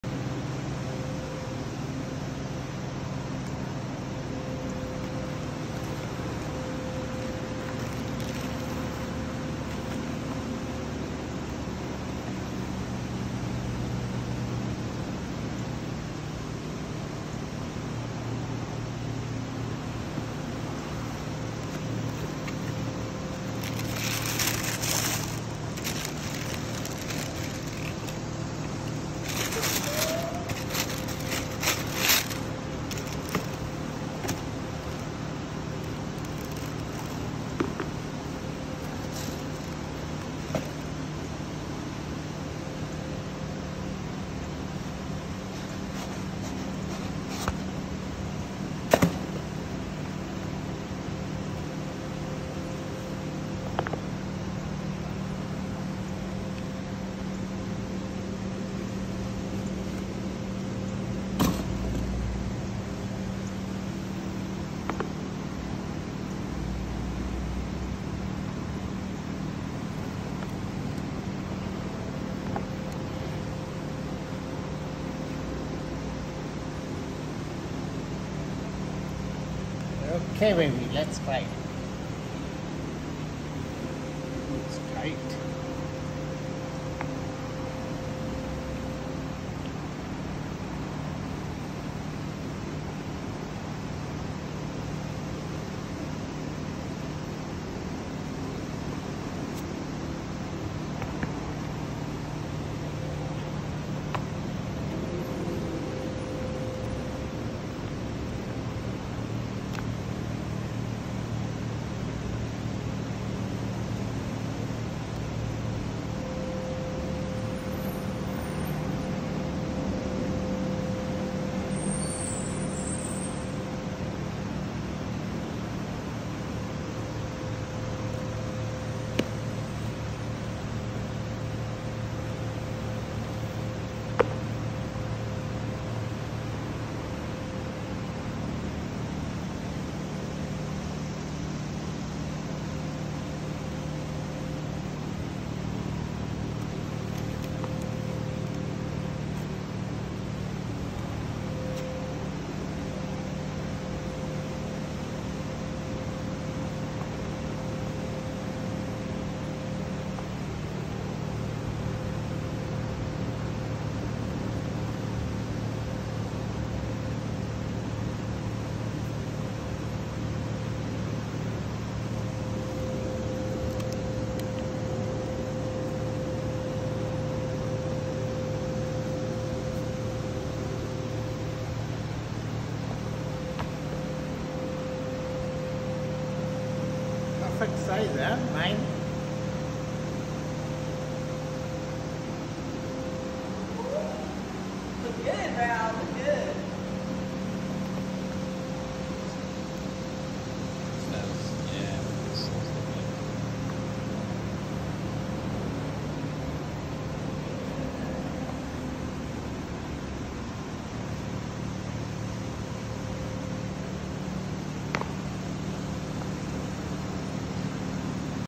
Vivobarefoot Geo Court II unboxing
Fits like a glove. Very light.  Chose size 9.  Used in Gym and for slow jogging.
Will update my experience in 30 days.